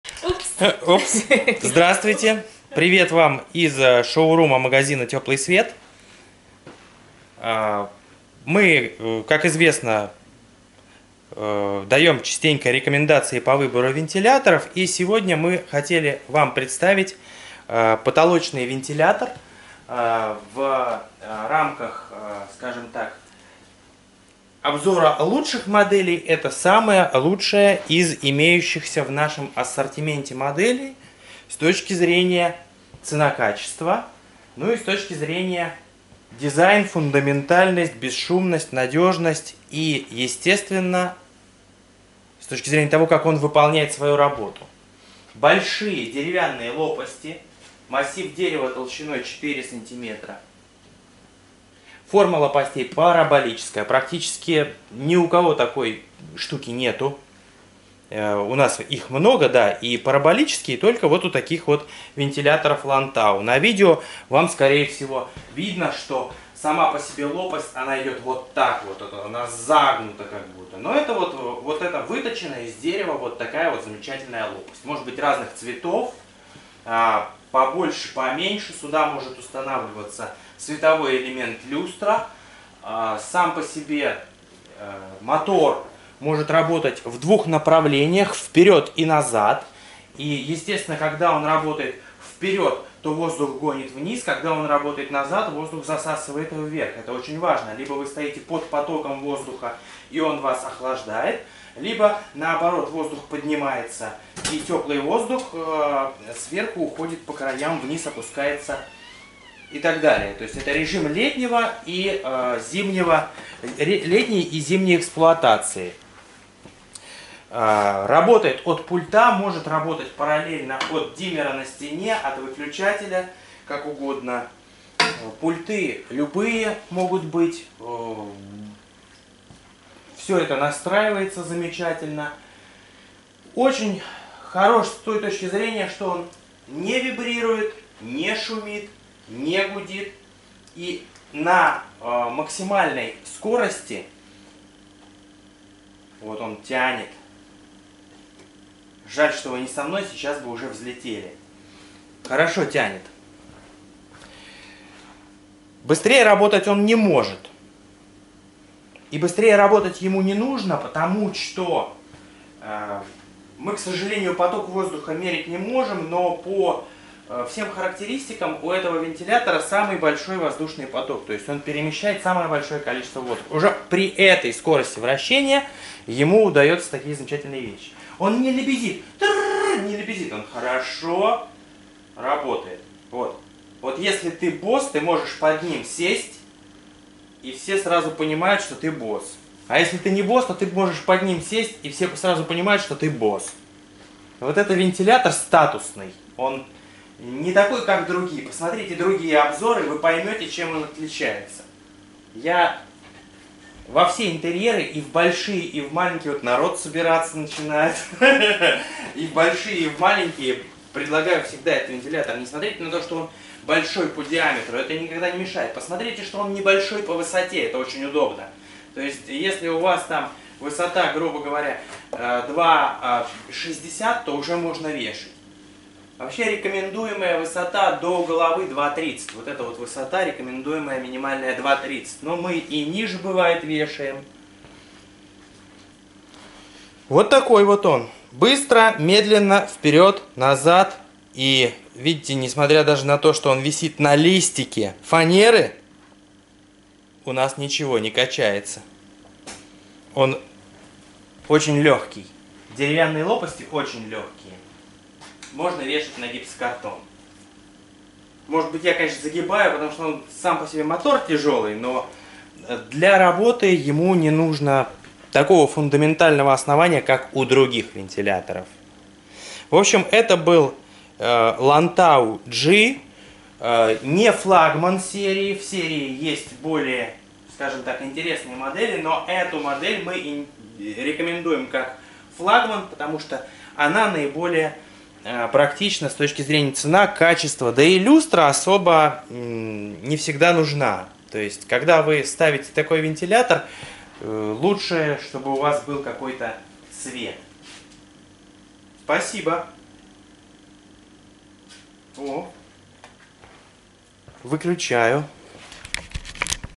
ХИТ! Люстра-вентилятор Faro LANTAU-G
Современный потолочный вентилятор с поразительным дизайном, идеально подходит для вентиляции помещений от 17,6 м2. Мотор из стали и деревянных лопасти. Питание от пульта дистанционного управления включено. Он имеет 3 регулируемые скорости с потреблением 25-50-90 Вт и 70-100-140 оборотов в минуту в зависимости от скорости. Этот вентилятор имеет обратную функцию, поэтому вы также можете использовать его зимой, повышая эффективность вашей системы отопления. Подходит для наклонной крыши.